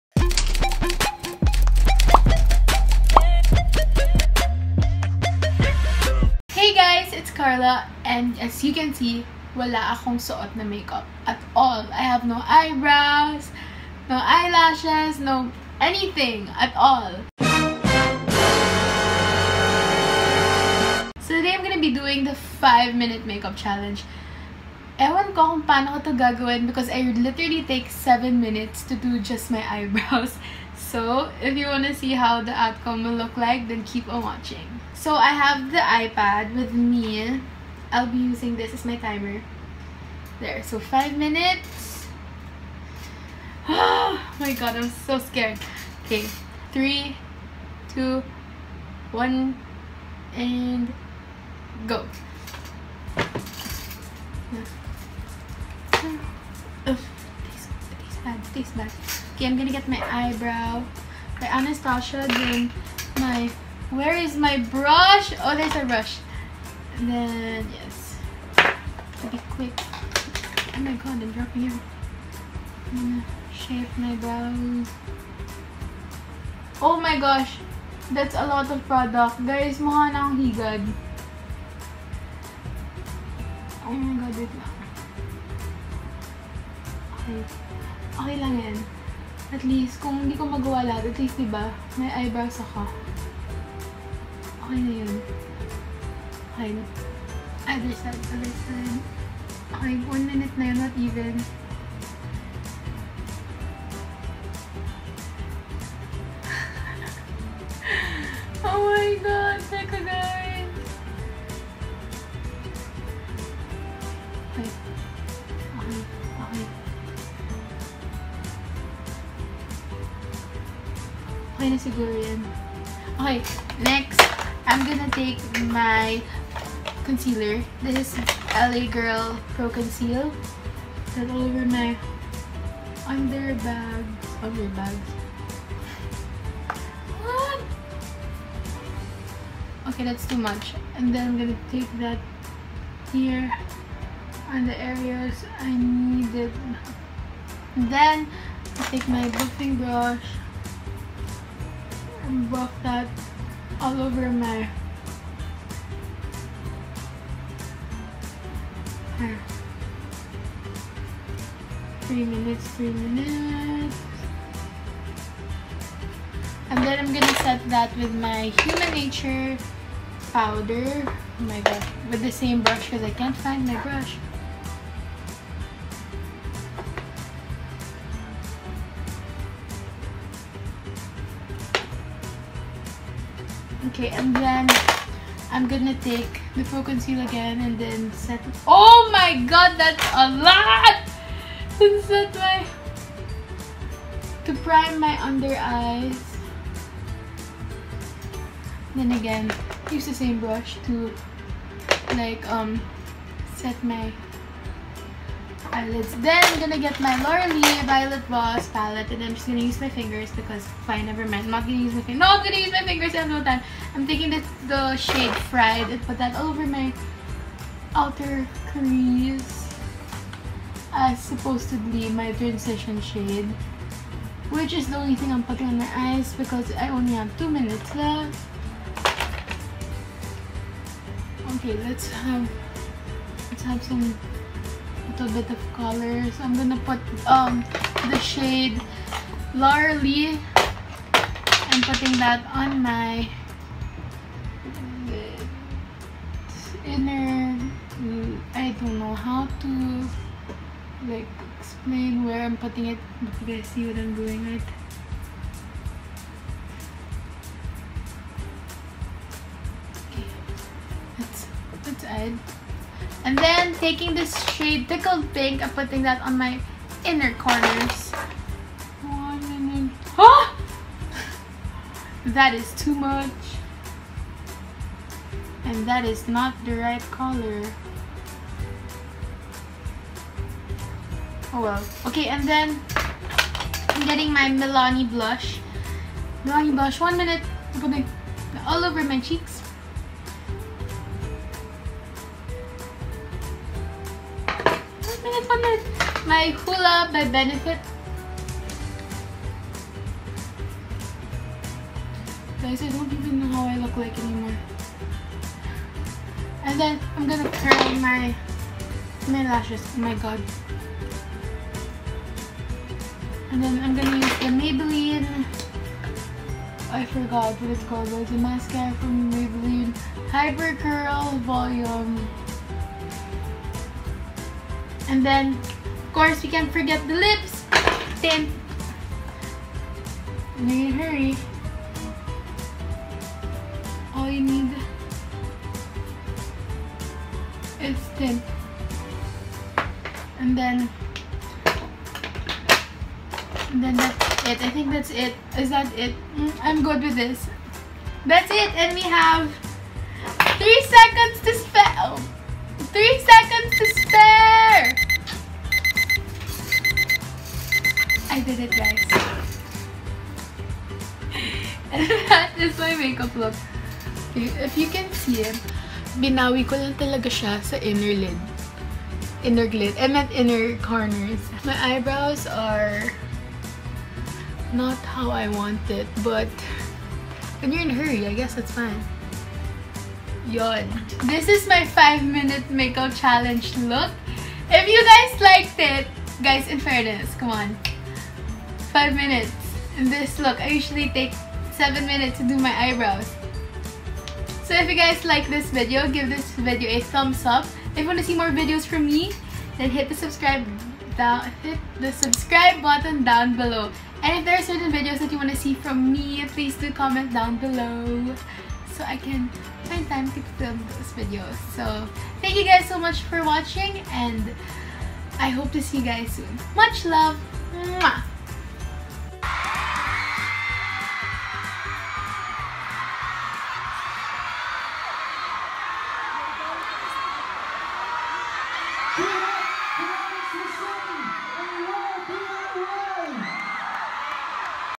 5 MINUTE MAKEUP CHALLENGE (philippines) | Karla Domingo
Hey guys! here's my version of the 5 minute makeup challenge. grabe hassle and nakaka pressure siya as in bes!!! HUHUHU